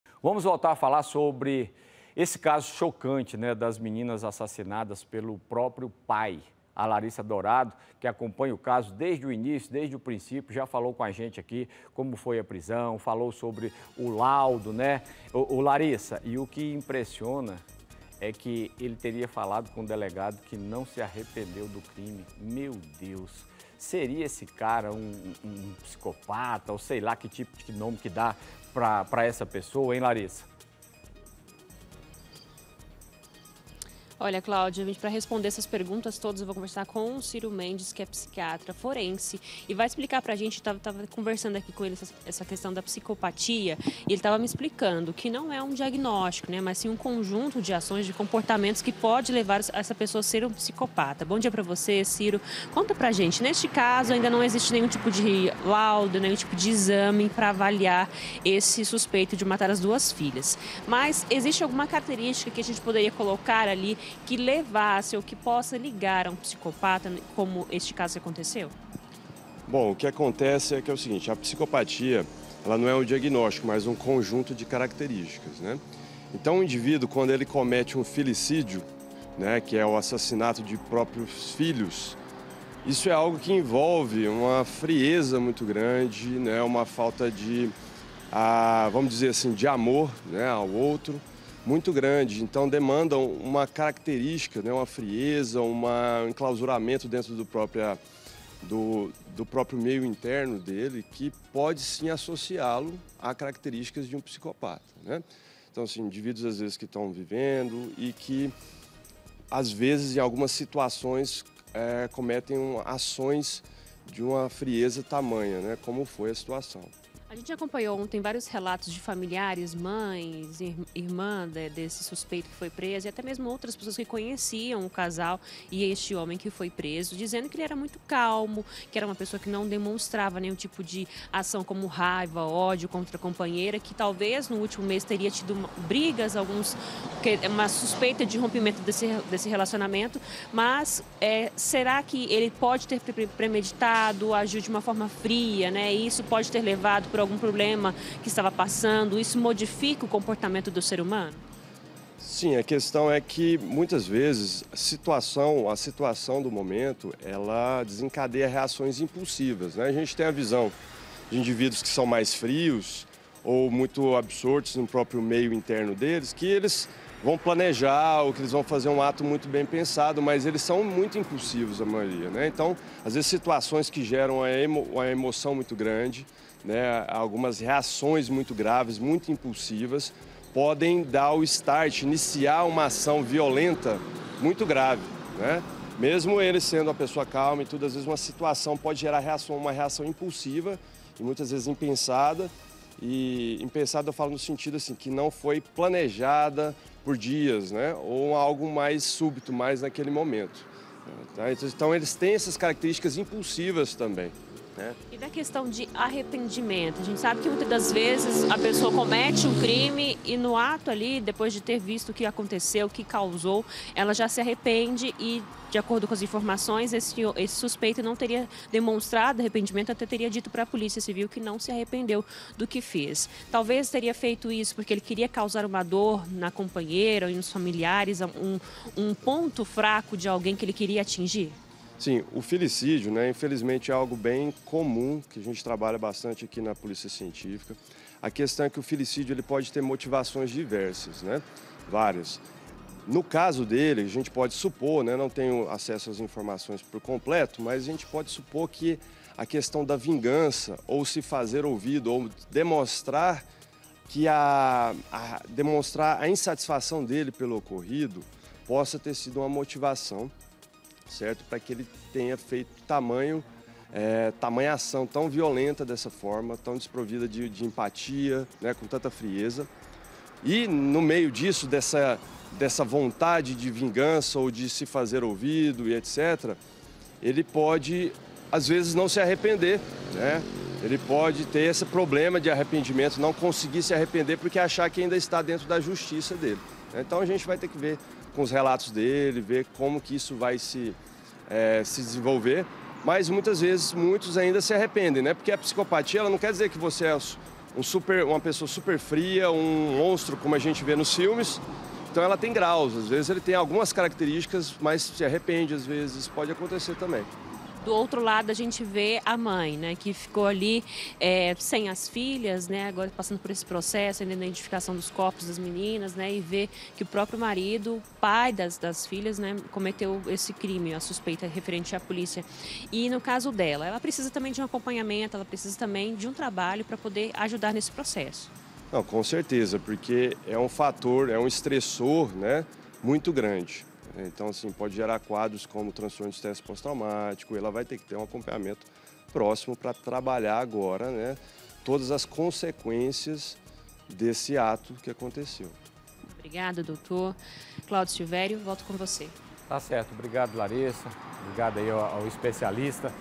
CRIANÇAS CARBONIZADAS: HOMEM NÃO TERIA SE ARREPENDIDO DO CRIME
VOLTAMOS A FALAR SOBRE ESSE CASO CHOCANTE DAS MENINAS ASSASSINADAS PELO PRÓPRIO PAI. LARISSA DOURADO ACOMPANHA O CASO DESDE O PRINCÍPIO, JÁ FALOU AQUI COM A GENTE COMO FOI A PRISÃO, FALOU SOBRE O LAUDO. LARISSA, O QUE ME IMPRESSIONA É QUE ELE TERIA FALADO COM O DELEGADO QUE NÃO SE ARREPENDEU DO CRIME. SERÁ QUE ELE É UM PSICOPATA? OU QUE TIPO DE NOME A GENTE DÁ PRA UMA PESSOA DESSAS?